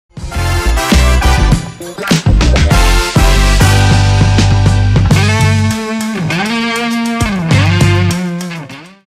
Grand Opening | Hyderi Karachi
Join us at the Grand Opening of Insignia out - Hyderi, North Walk Mall, Nazimabad Karachi.